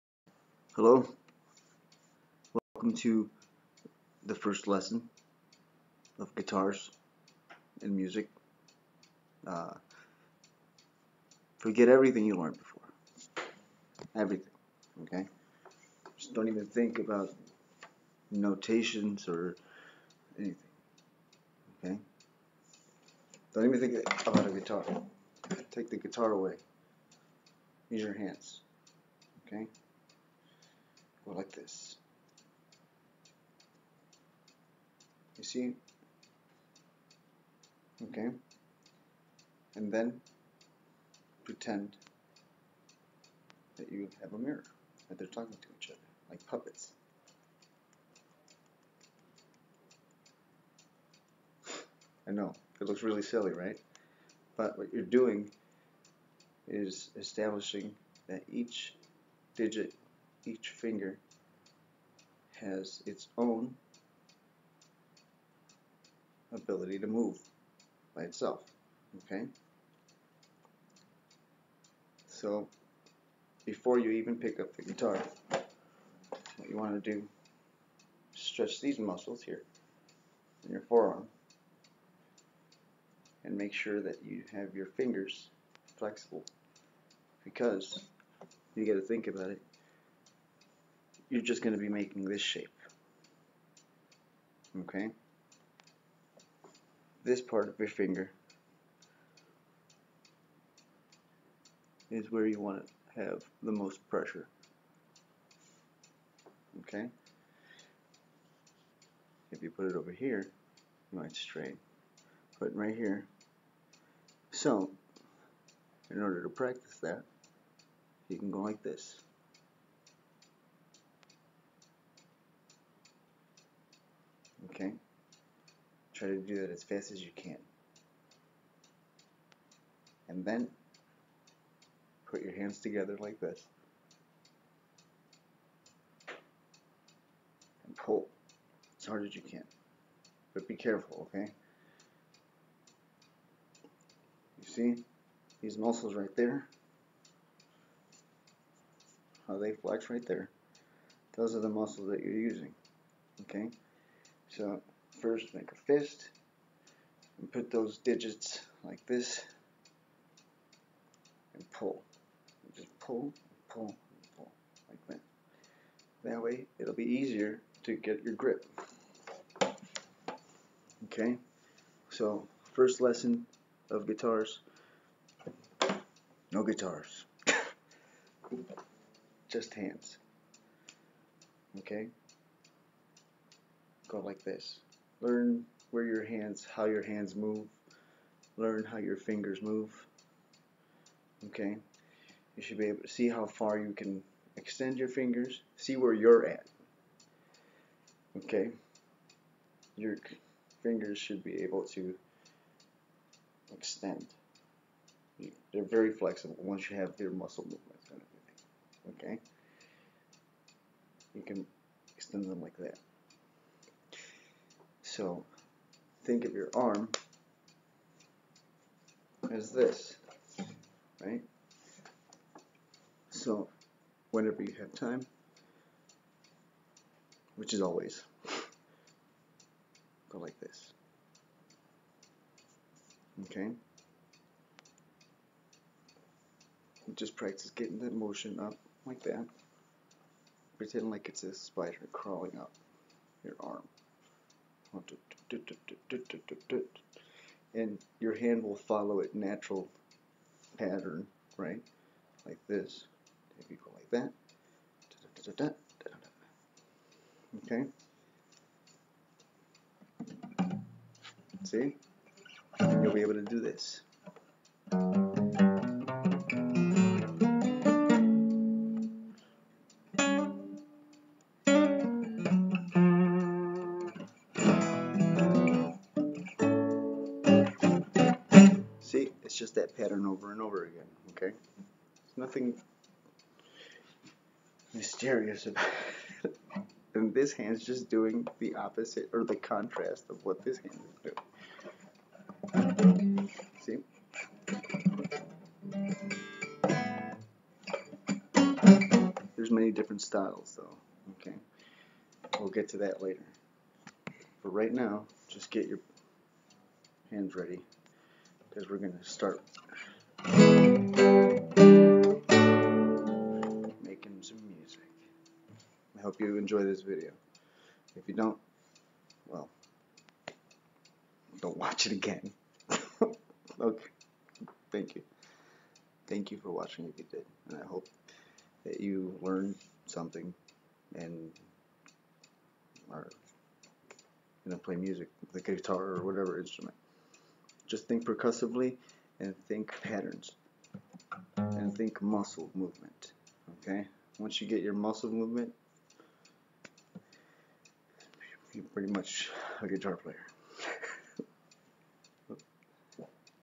GUITAR PLAYER EXERCISE
First lesson in playing the guitar. don't play the guitar! play with your hands first. Explore how your hands and fingers move.